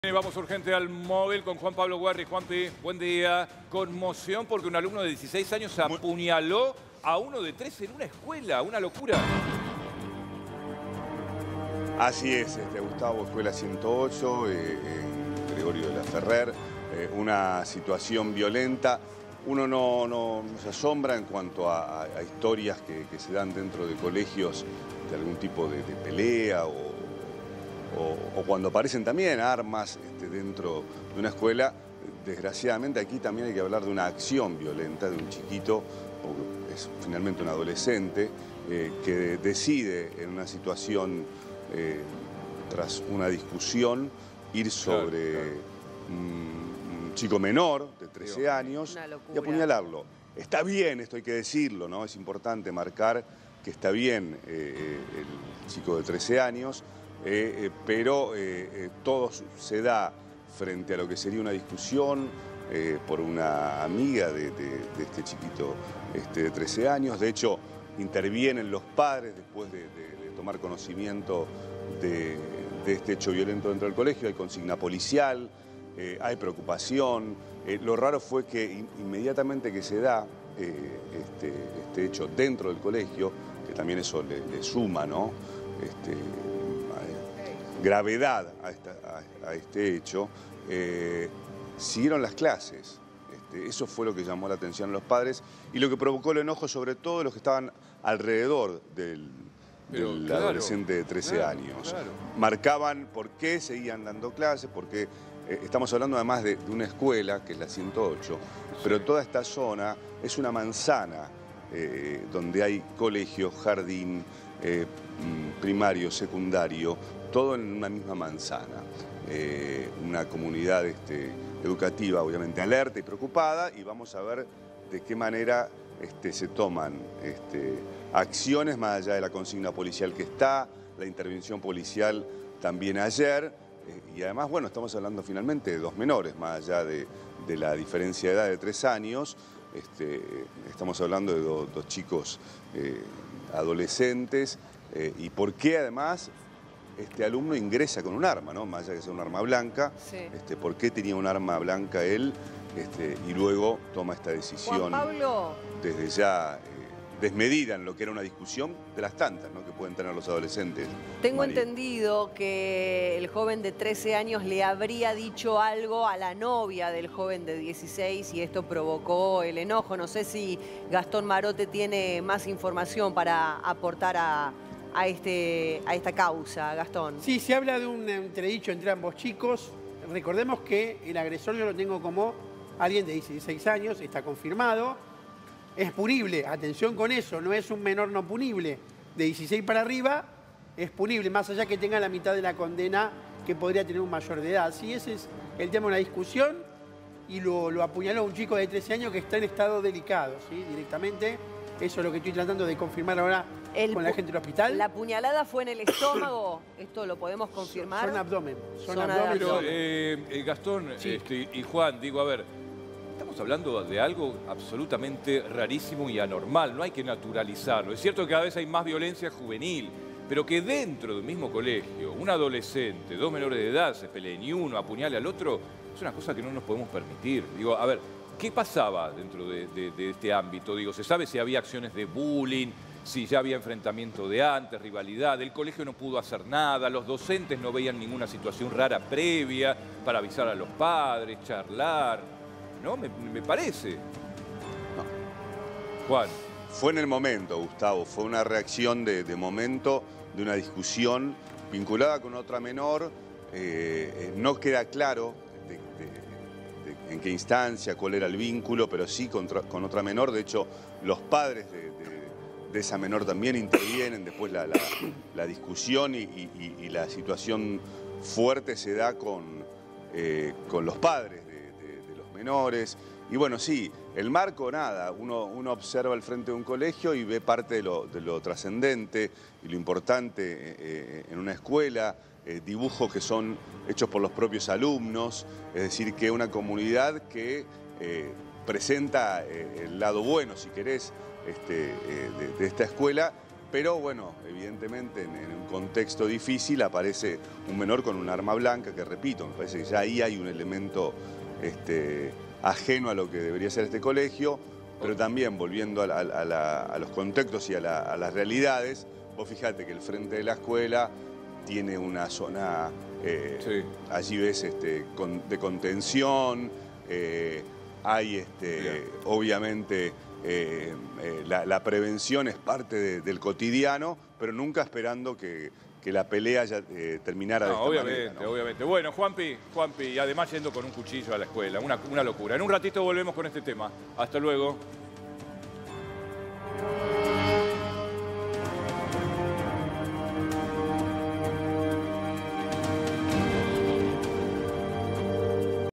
Tremendo: apuñaló a compañerito en el colegio y las clases continuaron como si nada
El brutal episodio tuvo lugar en la Escuela 108 de la localidad de Gregorio de Laferrere.

Según trascendió, un adolescente de 16 años tomó la decisión de atacar a otro en el marco de una discusión menor.

Un equipo de Crónica se acercó al lugar para contarte todos los detalles.

Crónica, el canal líder de noticias de la Argentina, creador de un estilo informativo único con sus placas rojas.